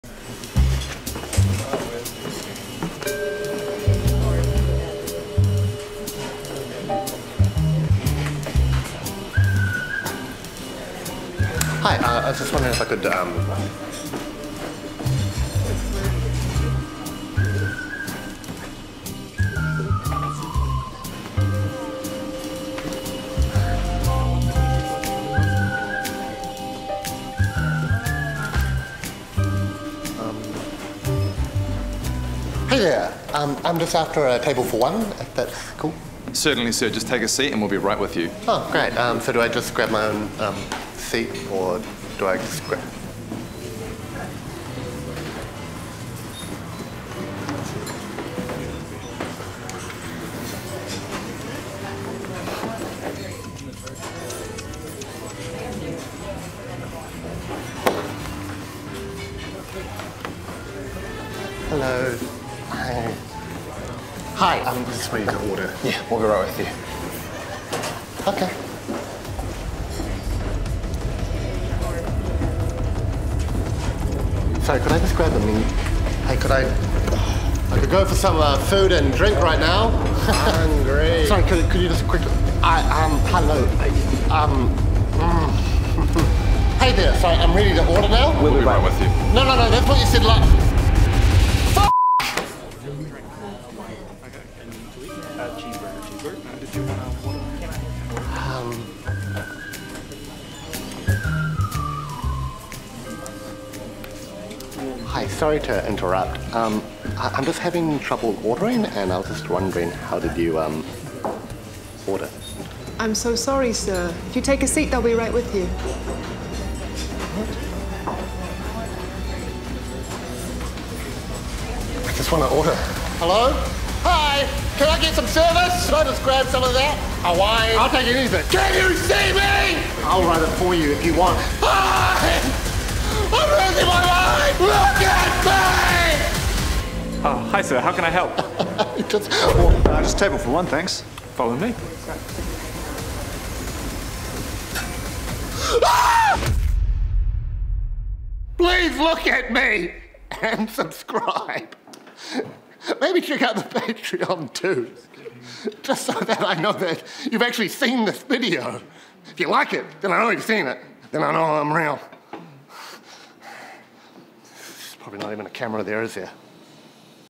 Being ignored in a restaurant
Everyone has experienced being ignored in a restaurant... but not quite as much as Hamish